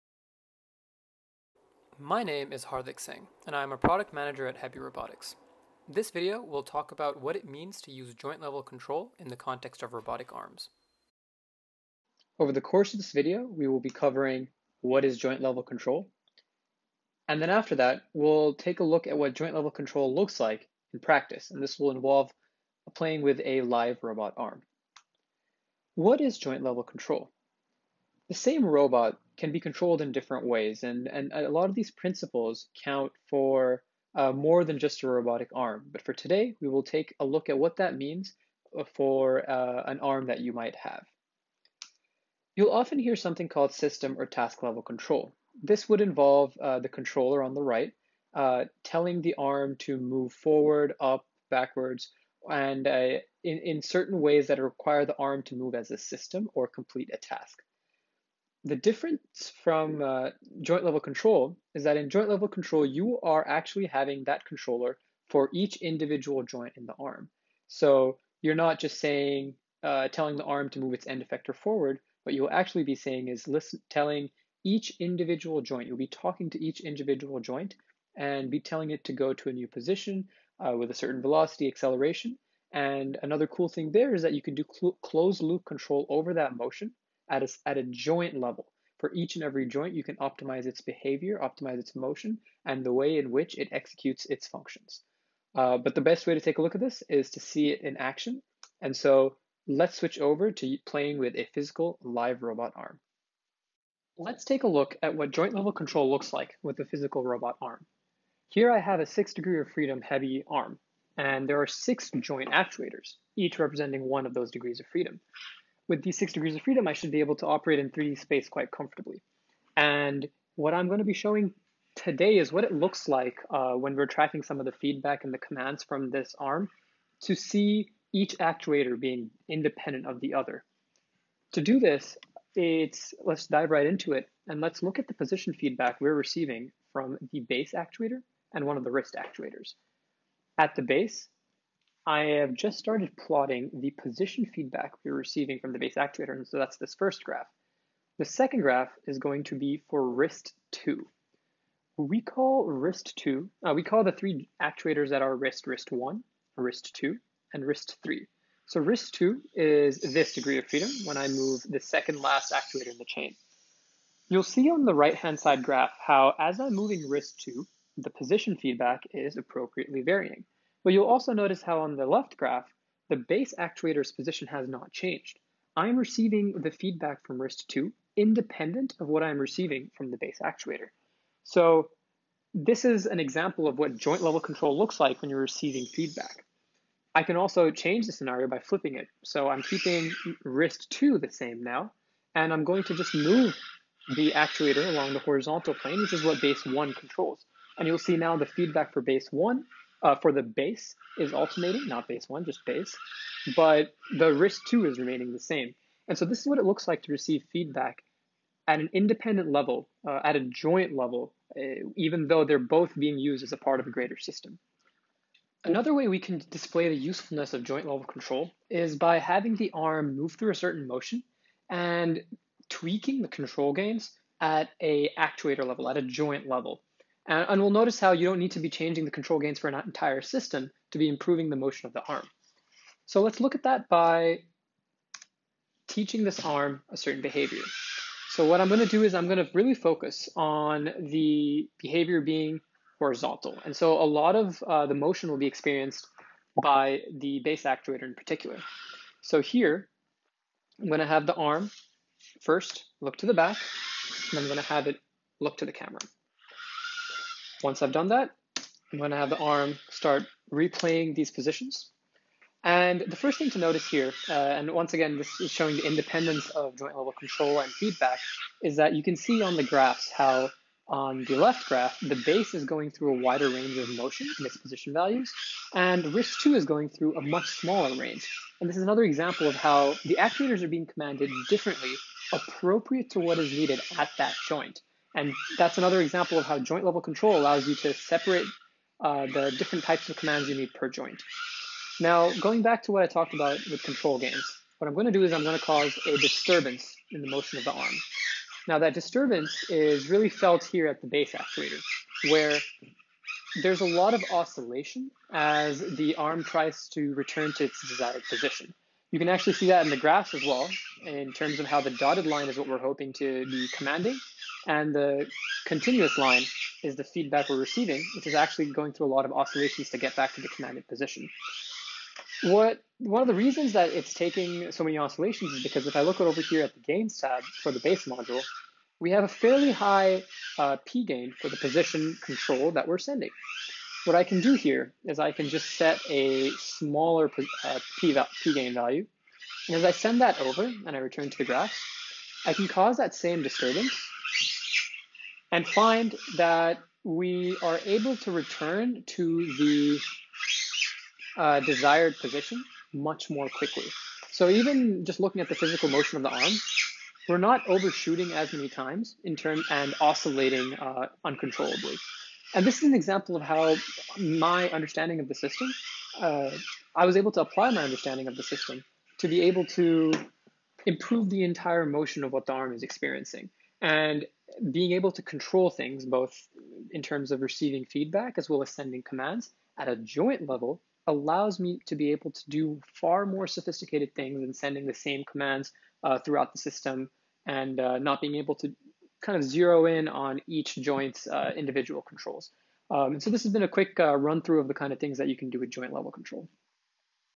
8. Robotic Arms - Joint Level Control (IROS 2020 Tutorial Series)
This tutorial series was created for the IROS 2020 conference, which was a free to access On-Demand Conference. The tutorial was titled "Introduction to Robotics Actuation and Motion Control".